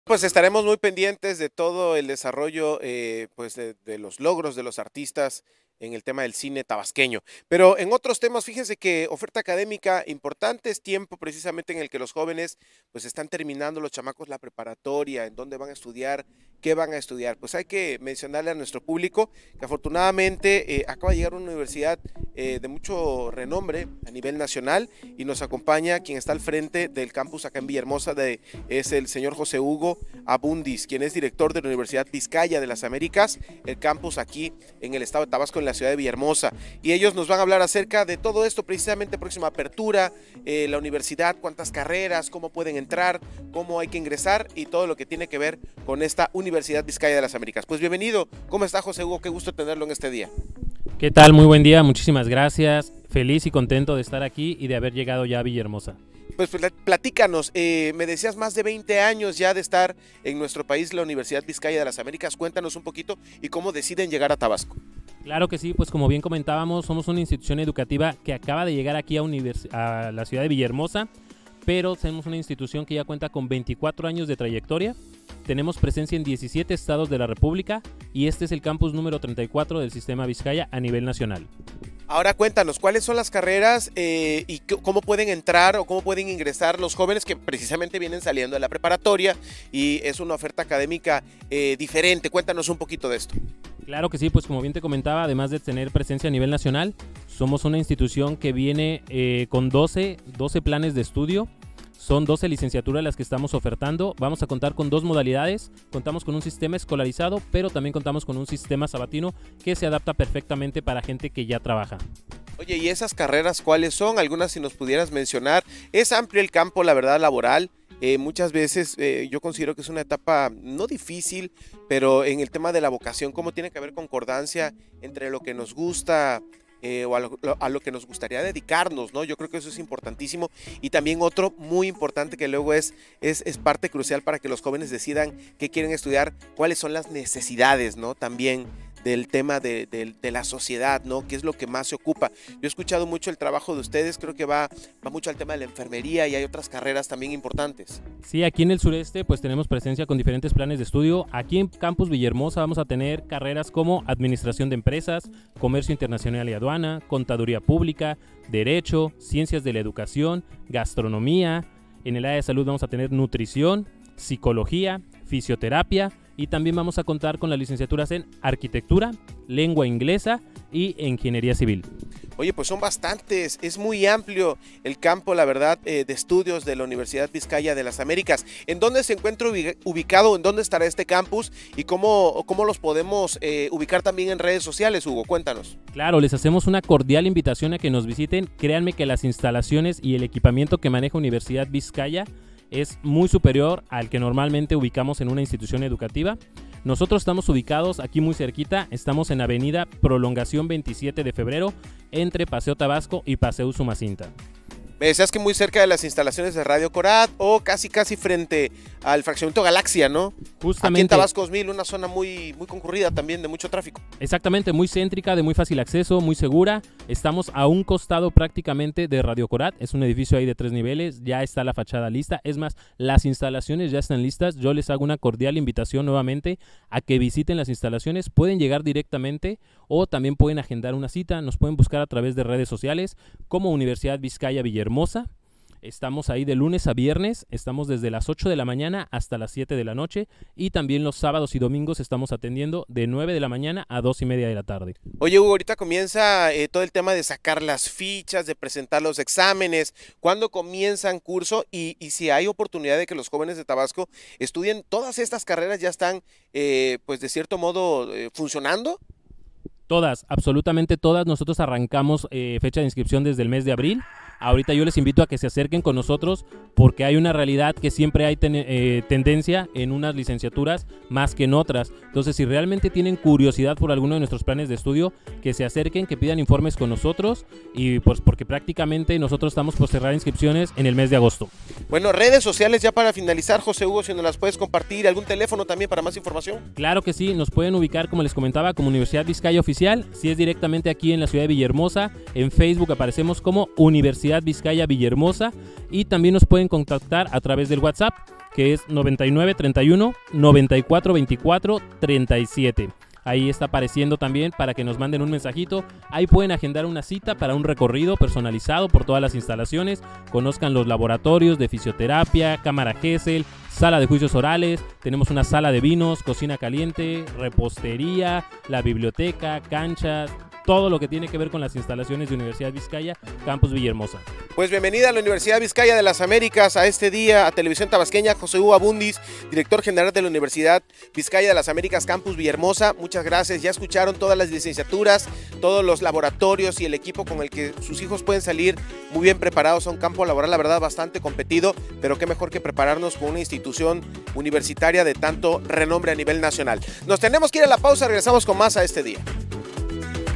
Entrevista con el Director General de Universidad Vizcaya de las Américas José Hugo Abundiz
Hoy en EsteDía 🌞 nos acompañó el Director General de Universidad Vizcaya de las américas (UVA) José Hugo Abundiz, quien nos habló del tema “Apertura del campus”.

Si quieres saber más, sintoniza al canal 46.1 Televisión Tabasqueña ¡Donde Lo Bueno Pasa! ✨